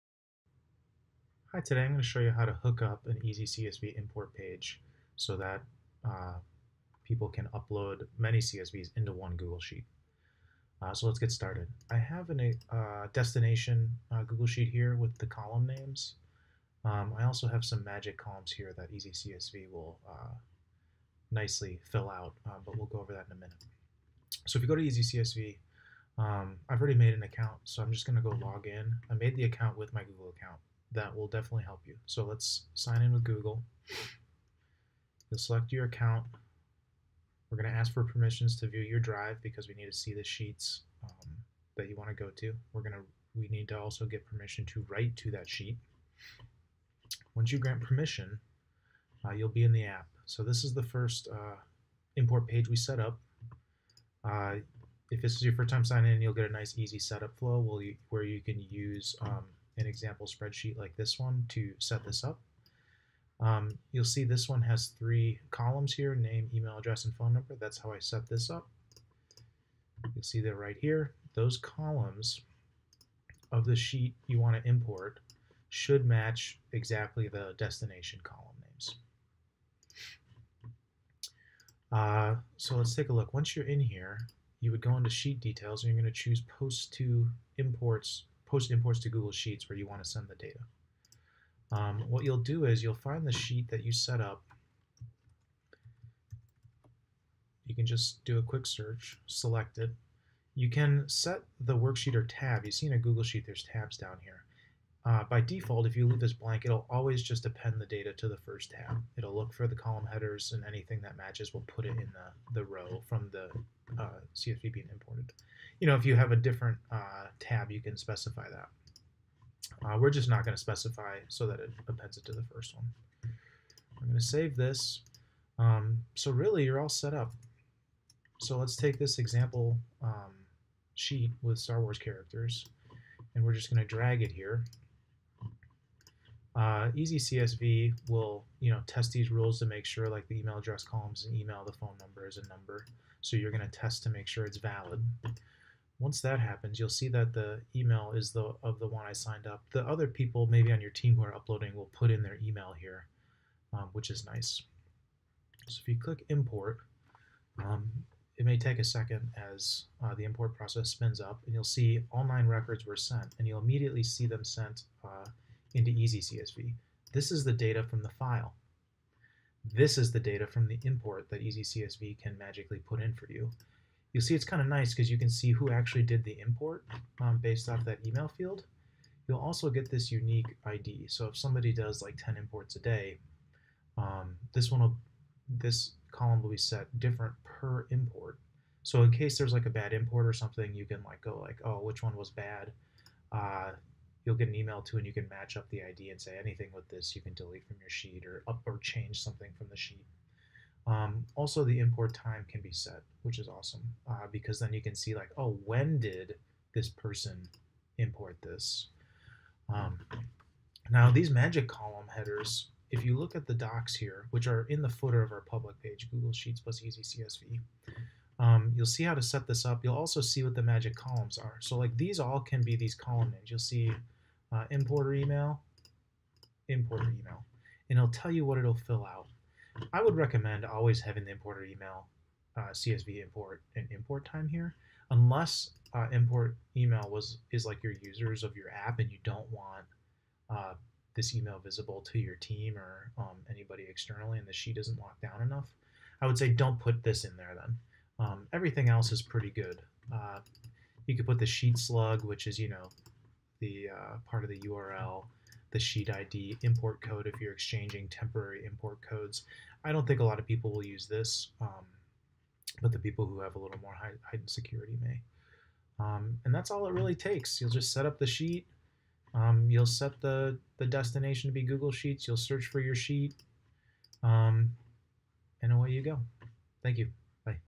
How to upload many CSVs into one Google Sheet - EasyCSV.io
Steps to use to upload many CSVs into one Google Sheet.